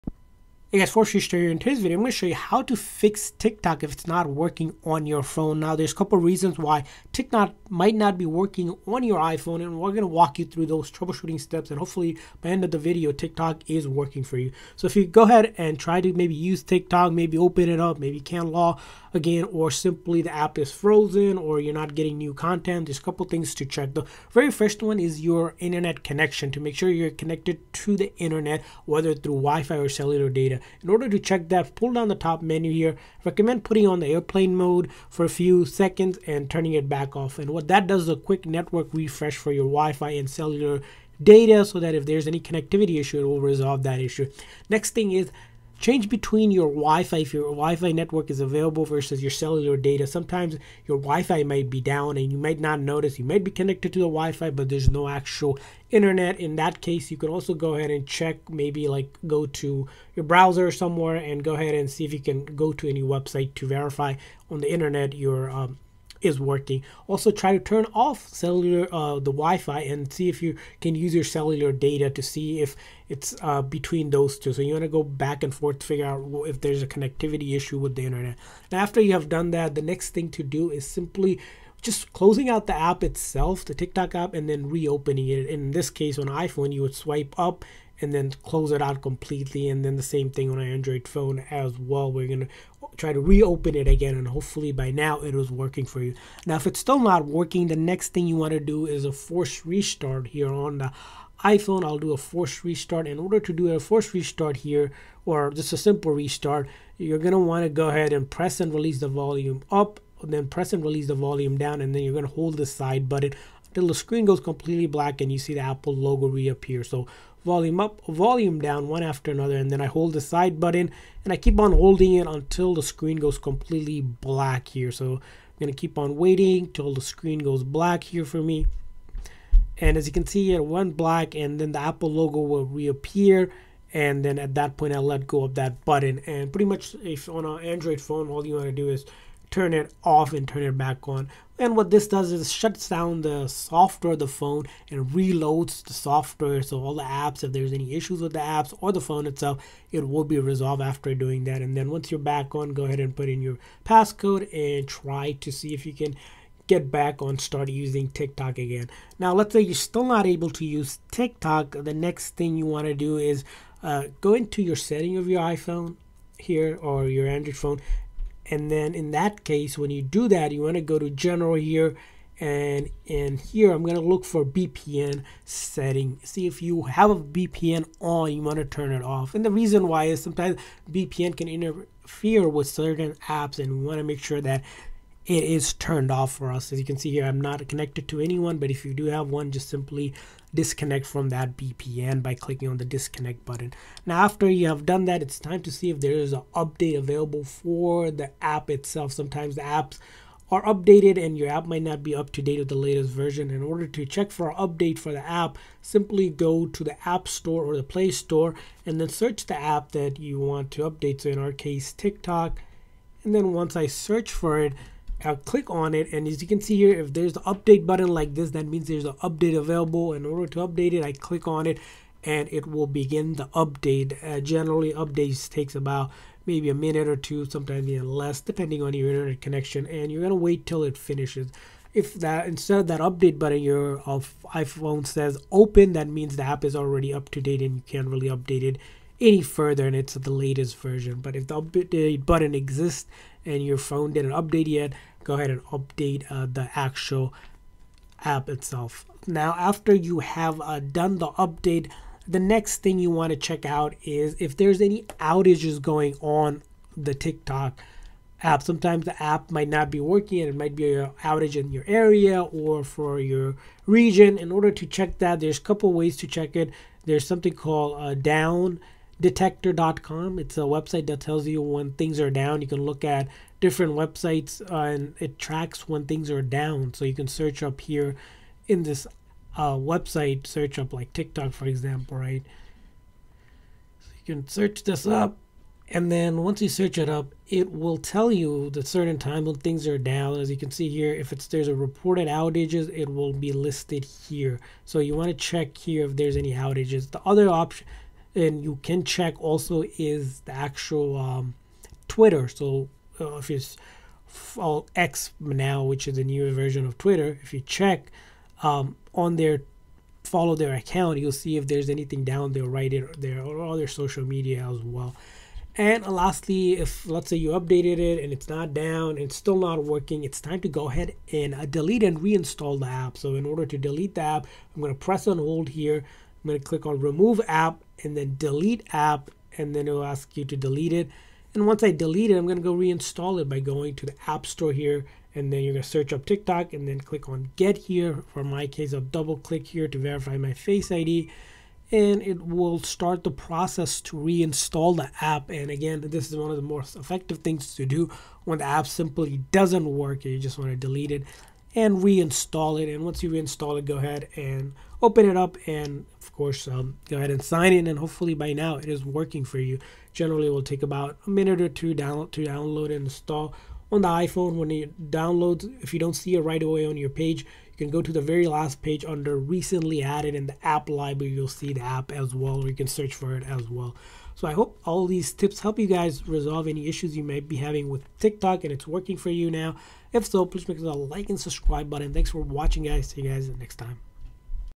TikTok Not Working? Here’s How to Fix It!
TikTok is a popular social media app that allows users to create and share short videos. However, sometimes TikTok can stop working for no apparent reason. This can be frustrating, especially if you're trying to create or watch videos. In this video we walk you though the troubleshooting steps in order to fix TikTok app not working!